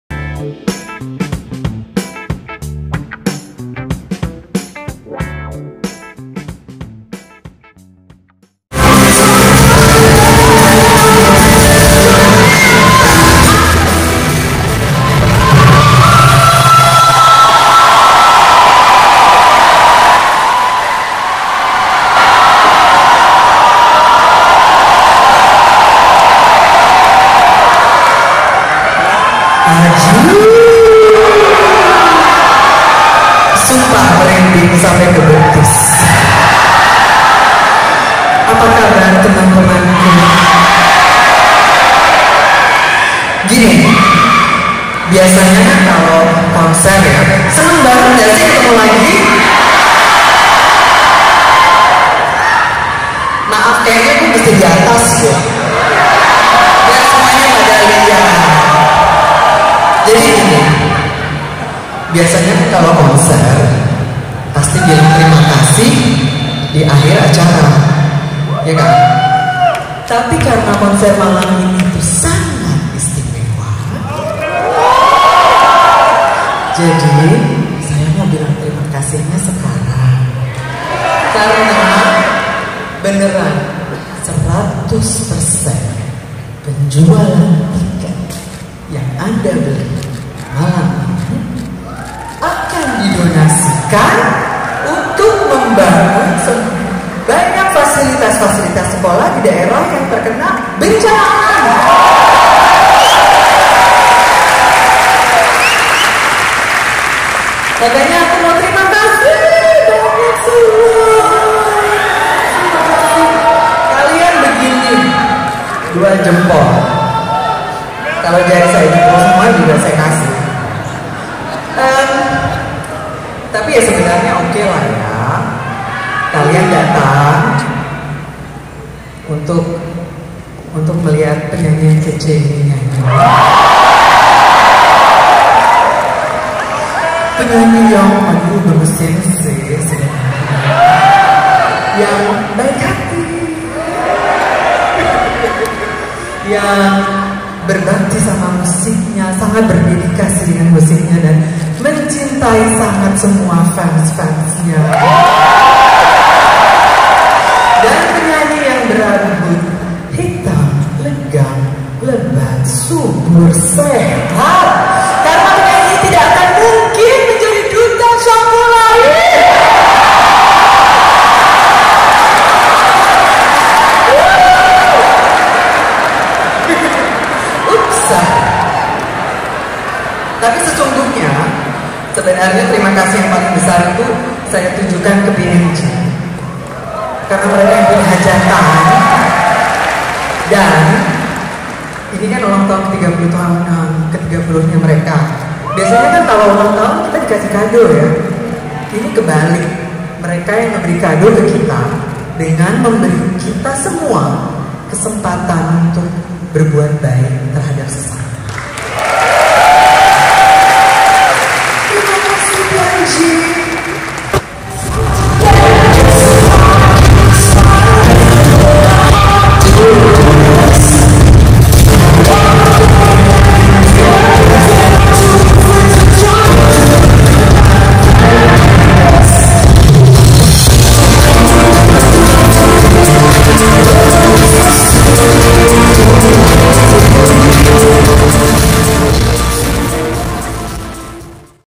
Anggun C Sasmi Konser Untuk Amal
Penyanyi Anggun C Sasmi bersama P&G menggelar konser di Tennis Indoor, Senayan, Jakarta. Seluruh pendapatan dari konser ini disumbangkan untuk membangun sarana pendidikan di lokasi yang terkena bencana alam.